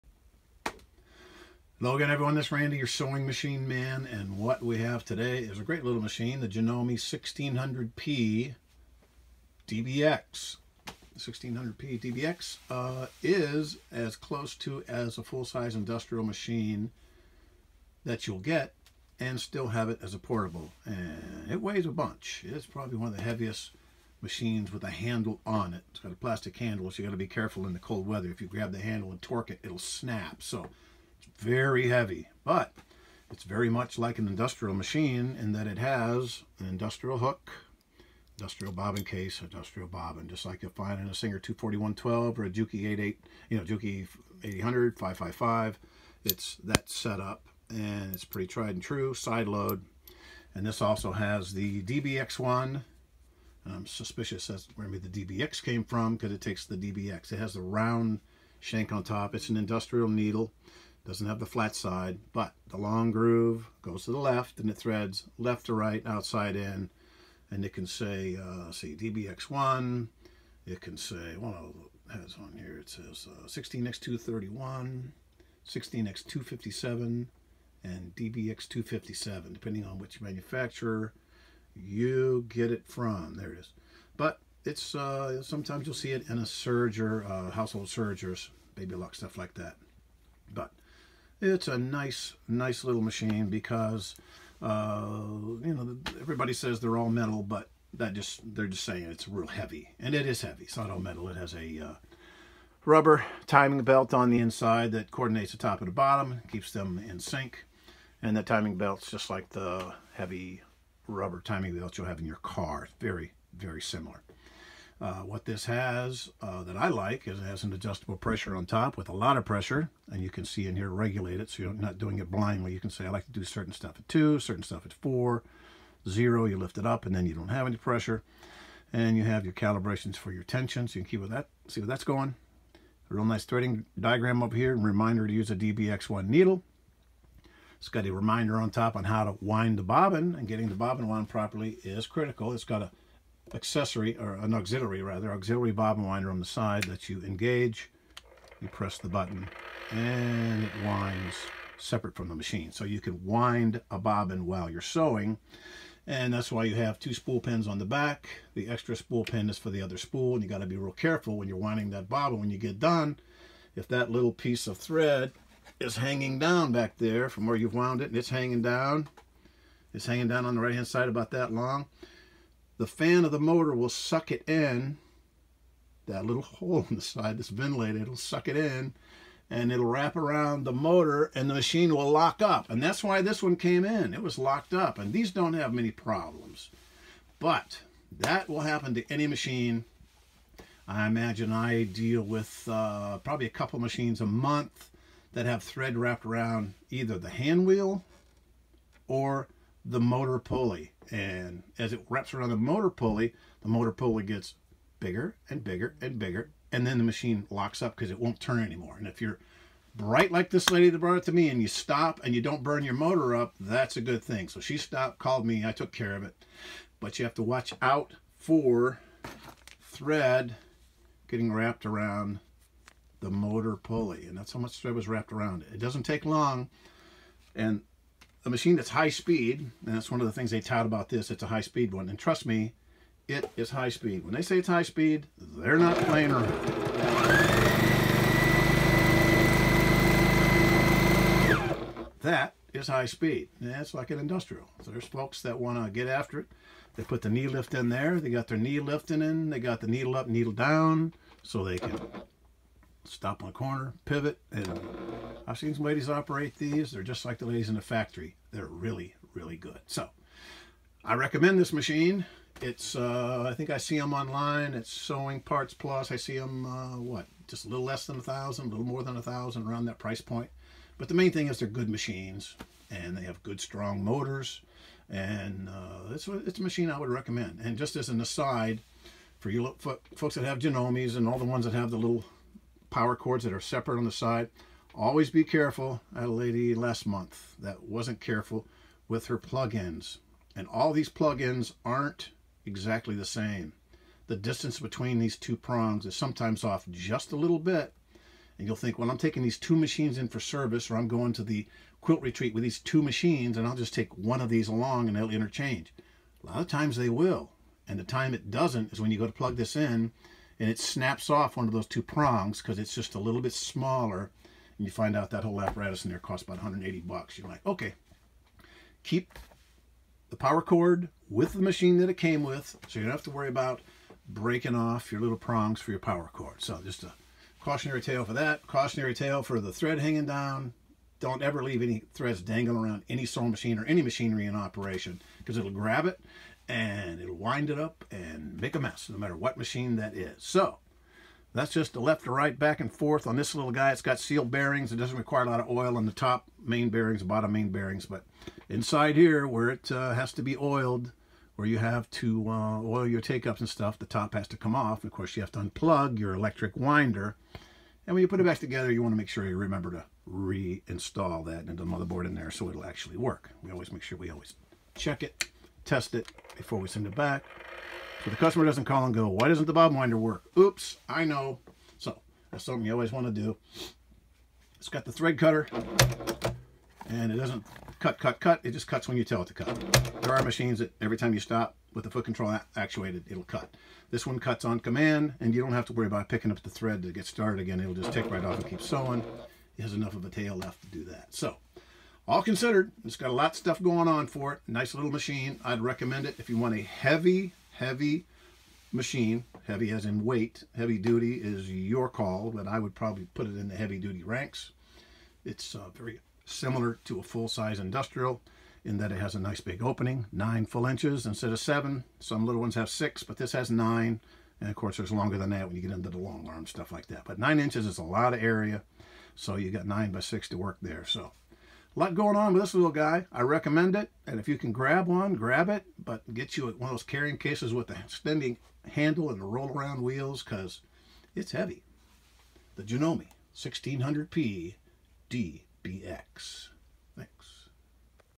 Janome 1600-P DBX revue.2 thumbs up- Industrial hook & bobbin case. DBx1 needle, Get one (Video 174)
Here’s a machine that I’m a big fan of. Many of my customers have them and like them too. Heavy as the dickens, but that’s what quality machines are like. If you’re starting an online business and don’t have the space for a full sized industrial machine in the 2x4 foot power stand, here’s a machine to consider. ((Video 174)